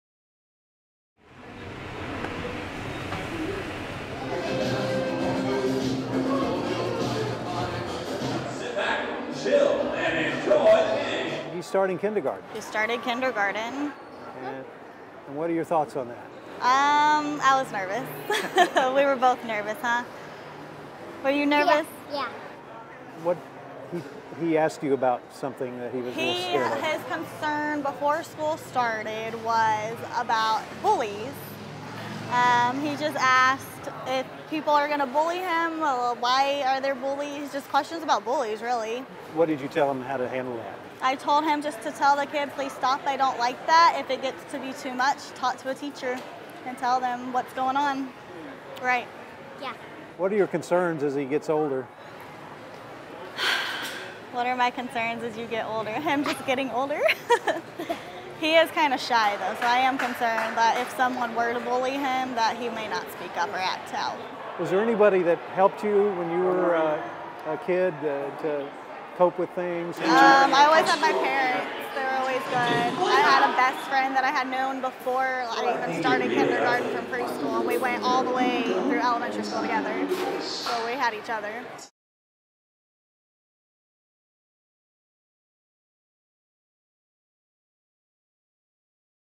Interview: Brittany Knedlik
As part of our “Mobile Newsroom” at a recent Round Rock Express baseball game to tell fans about our new “Save Our Students” project, our anchors spoke one-on-one with people.